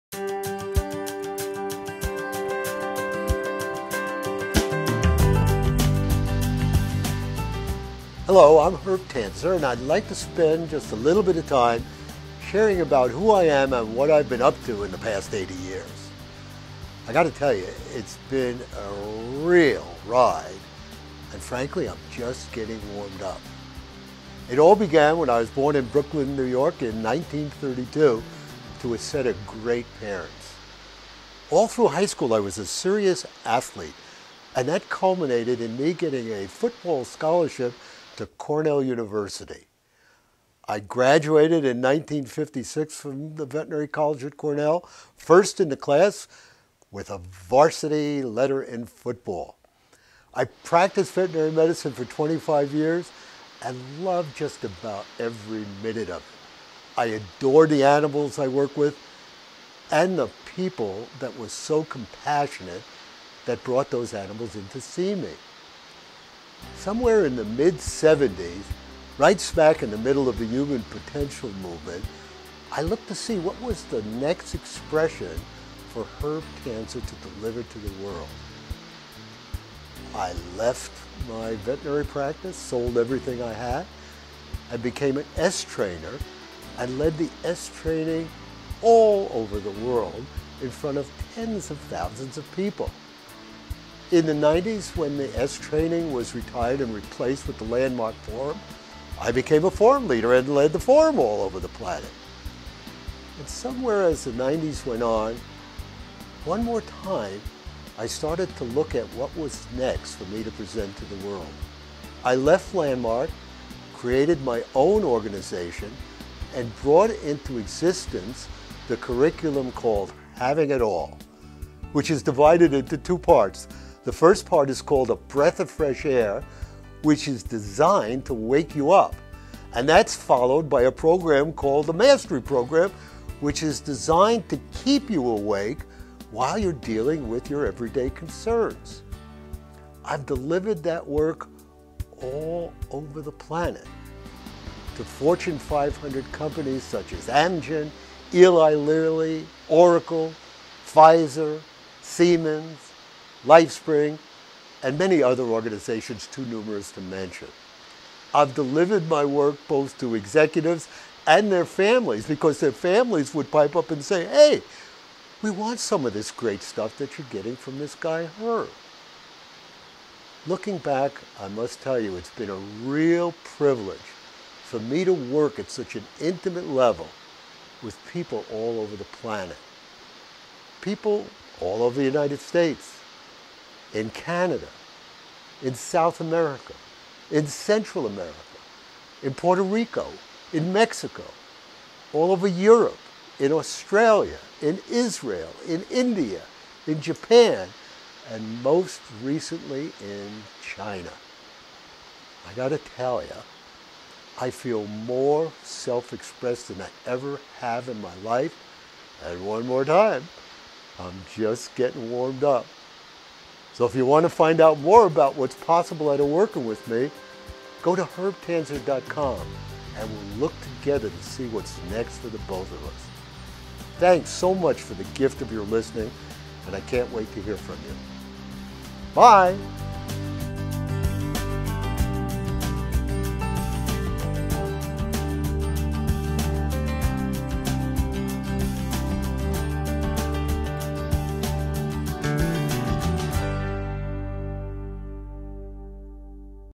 About Herb Tanzer
Watch this short 4 minute video to find out more about Herb Tanzer. Dr. Tanzer has an extensive background in assisting individual and groups to greatly enhance all aspects of of life. When you're complete, and surely wanting to know more, please visit the official and see a variety of other short videos.